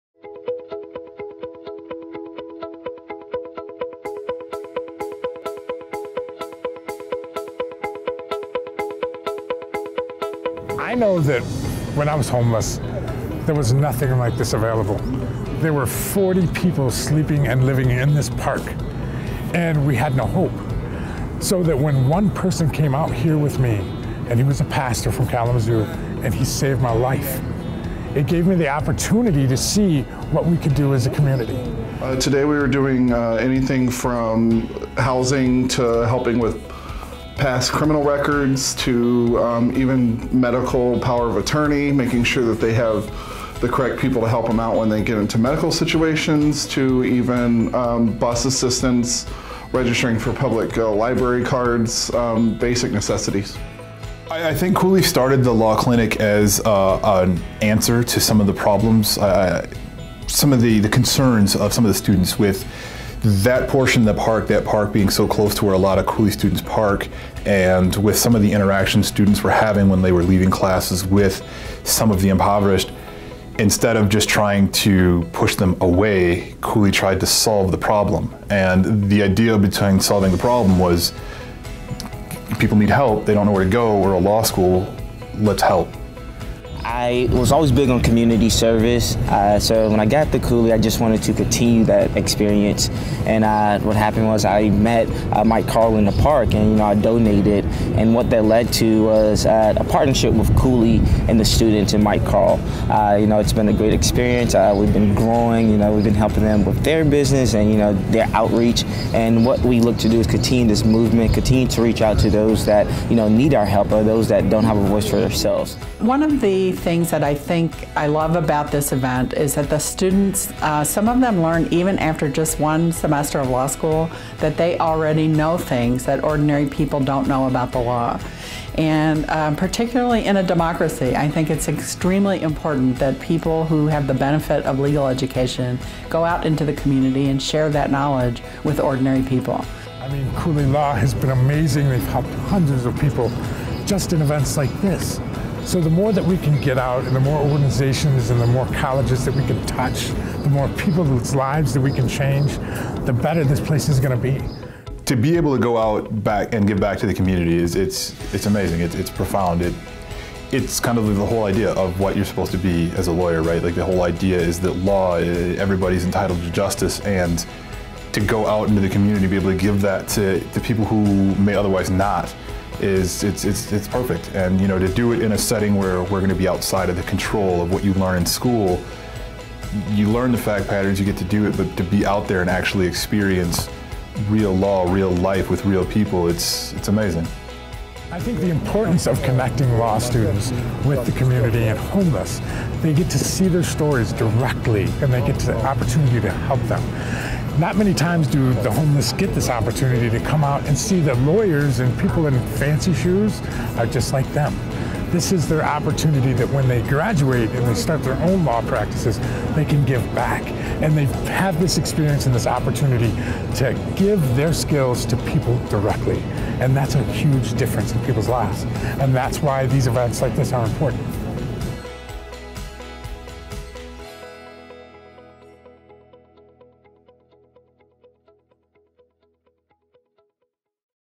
Cooley & Cardboard Prophets team up for ‘Street Law Stand Down’ to Help Lansing-Area Homeless
On Saturday, May 11, Cooley Law School Lansing campus students, faculty, staff, and alumni went to Reutter Park in downtown Lansing, Michigan, to offer legal assistance and other social services to the city’s homeless. The “Street Law Stand Down” clinic was held in collaboration with Lansing-based Cardboard Prophets, a street-based ministry and outreach organization that works with the homeless and families in need.